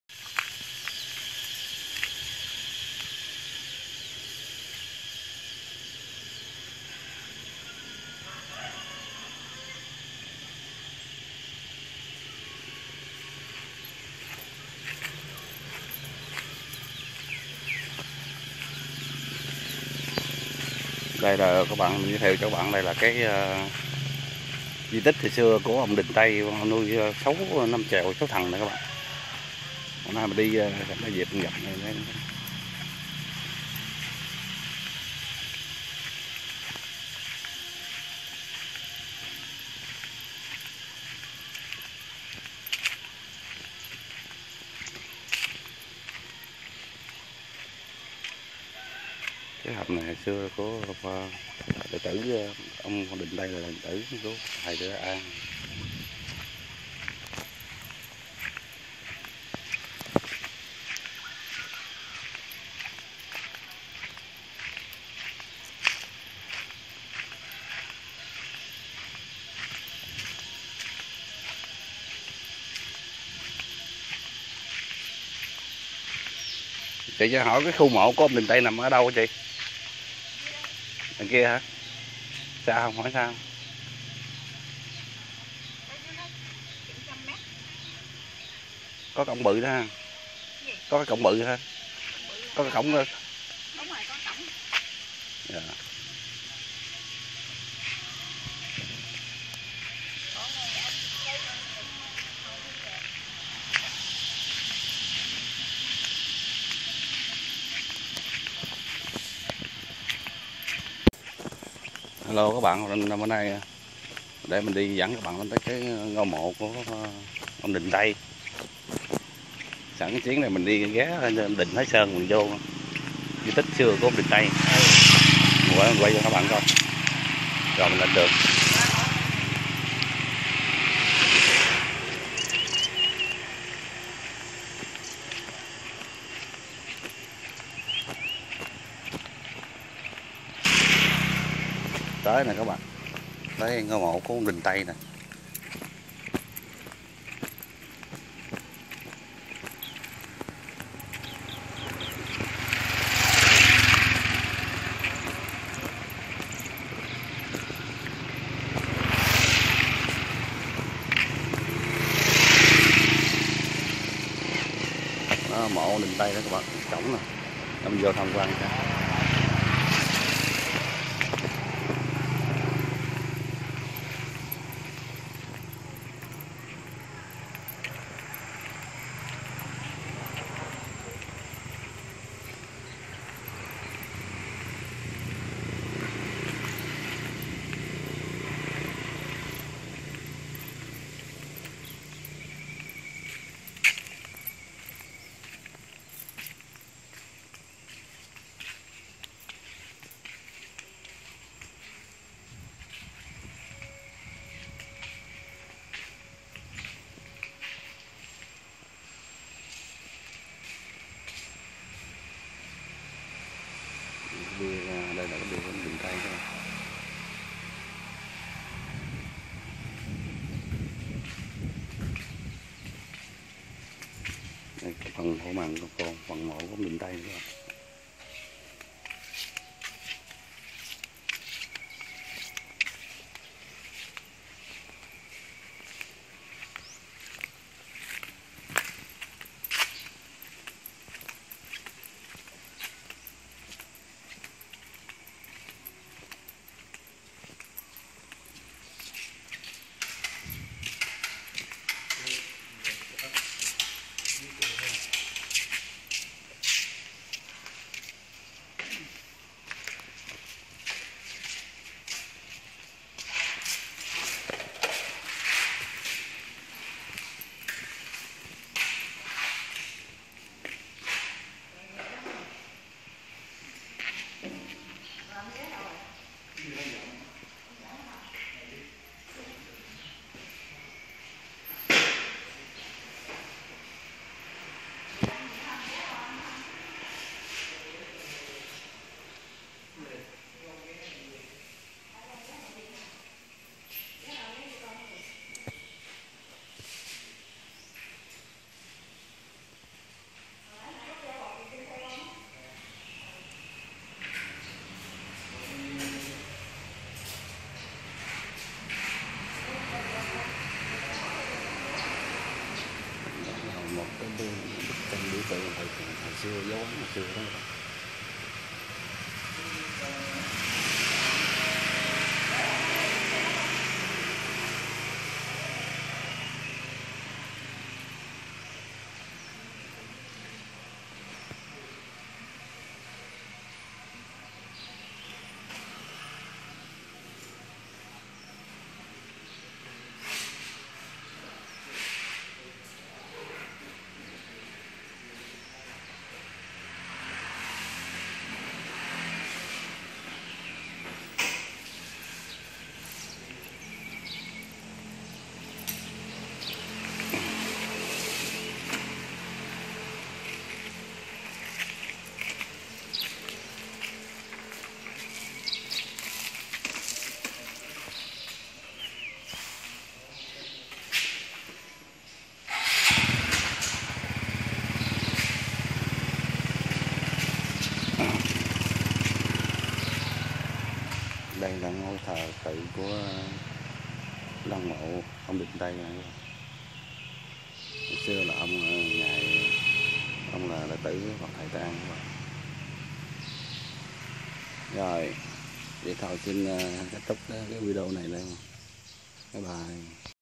khu di tích phật thầy tây/ông đình tây nuôi cá sấu thần
ngày xưa phật thầy tây an khai sáng đạo bửu sơn kỳ hương /và di tích cá sấu thần/xinh mời xem và để lại conmen cho mình biết /cảm ơn bạn nhiều